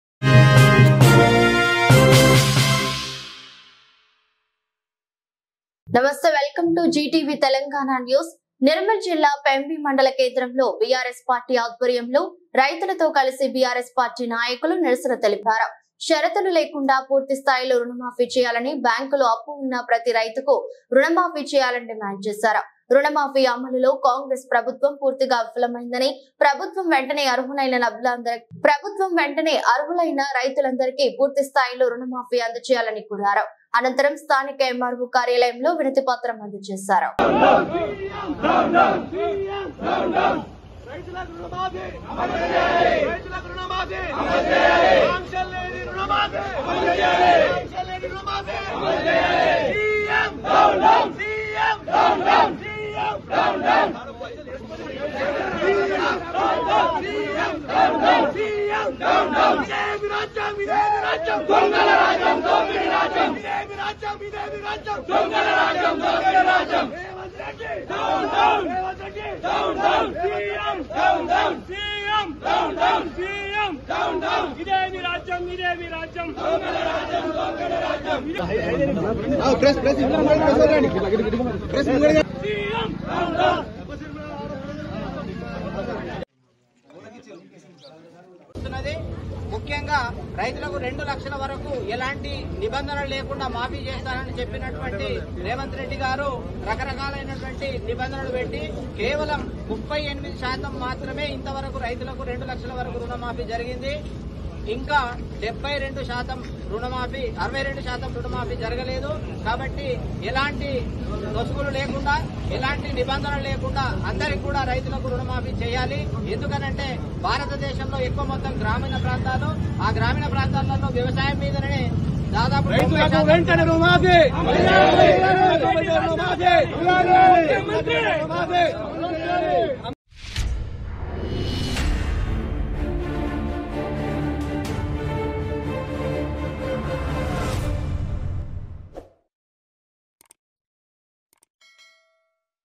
రైతులకు పూర్తిస్థాయిలో రుణమాఫీ చేయాలని పెంబి మండల కేంద్రంలో రైతులతో కలిసి బీఆర్ఎస్ నాయకుల నిరసన
రైతులకు పూర్తిస్థాయిలో రుణమాఫీ చేయాలని పెంబి మండల కేంద్రంలో రైతులతో కలిసి బీఆర్ఎస్ నాయకుల నిరసన @GTVTelangana

GTV TELANGANA
జనం కోసం జర్నలిజం .
DIGITAL MEDIA .

All political parties public meetings live telecasting this channel. GTV Telangana